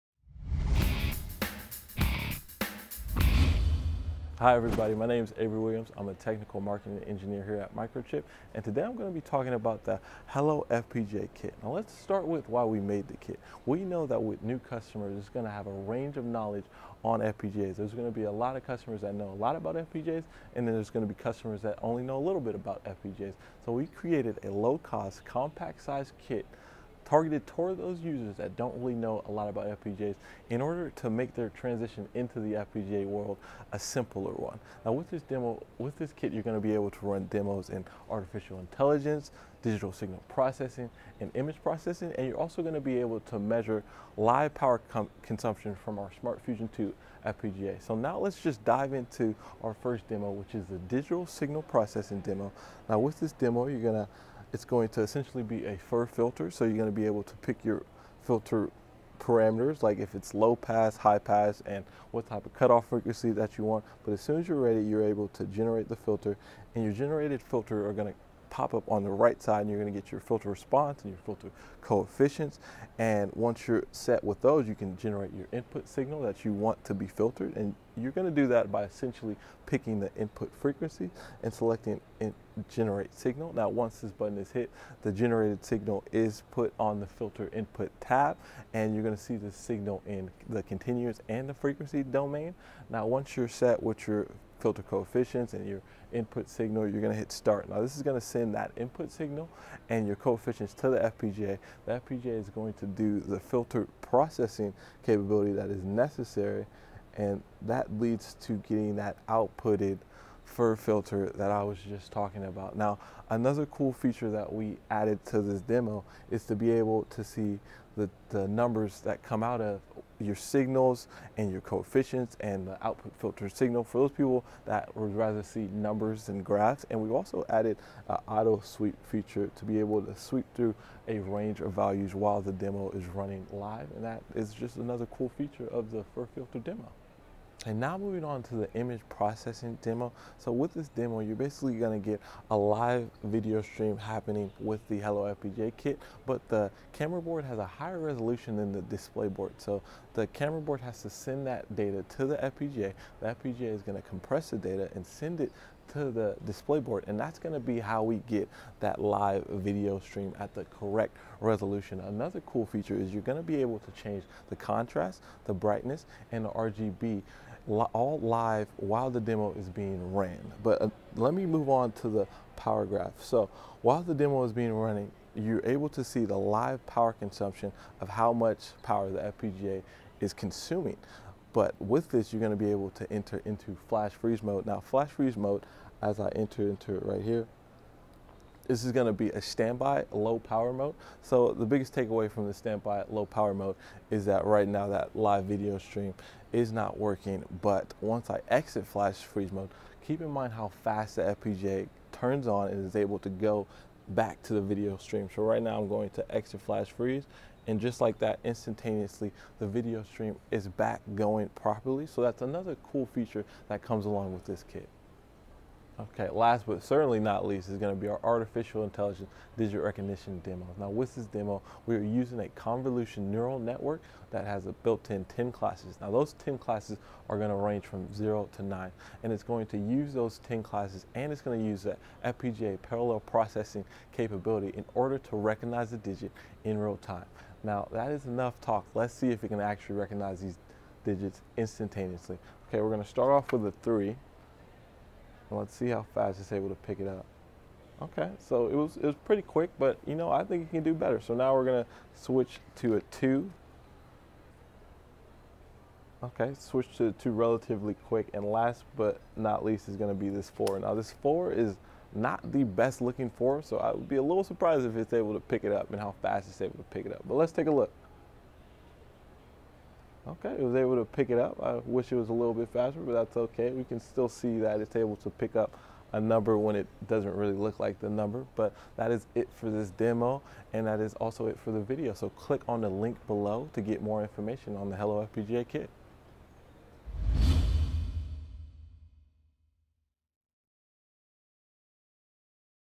Hello FPGA Kit Demonstration
Microchip's Hello FPGA kit is a entry-level platform targeted towards end-users with low to medium FPGA knowledge. See how the kit supports demos in Image Processing, Digital Signal Processing, Artificial Intelligence, and can even measure live power consumption while these demos are being ran.